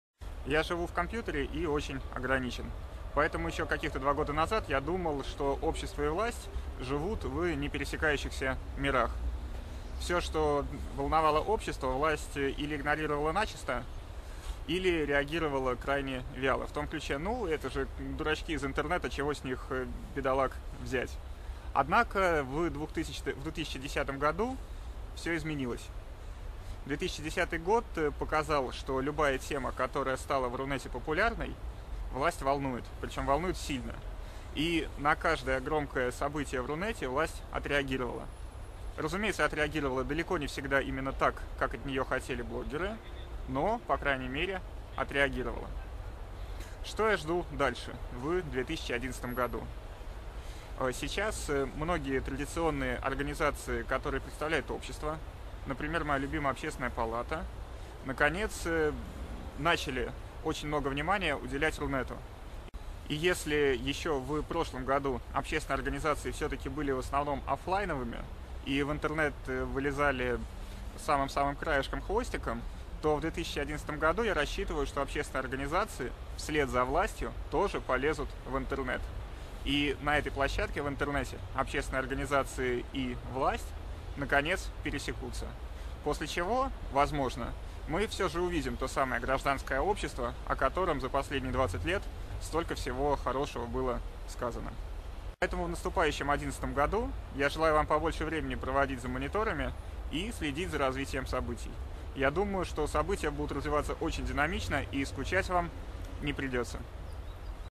Фриц Морген: блогеры меняют общество
Известный блогер Фриц Морген рассказал "Порталу НКО" о том, как он  оценивает соотношение сил в дуэте "власть - блогосфера" и какое место в этом соперничестве за информационное поле и умы людей занимают некоммерческие организации.